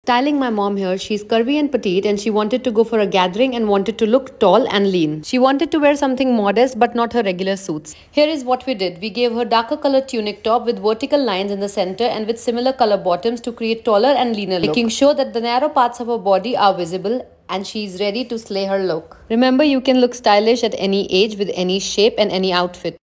Styling curvy and petite body structure
Having a style agenda is a must. Knowing what do you want to look and feel like should be the basis of your final outfit.

Vertical line tunic

Looking stylish is not about wearing sexy clothes, it's about wearing clothes that resonate with you.ight pants

Some style tips to for curvy and petite structure

Add vertical lines

Wear monochromatic looks

Add minimum heels

Show your narrow parts

Stick to high waisted

Remember you don't have to be of a certain Thadd the rook stylish. If you age or shape to look stylish. If you are looking to amp your style then it's a sign. DM TO START YOUR STYLE JOURNEY.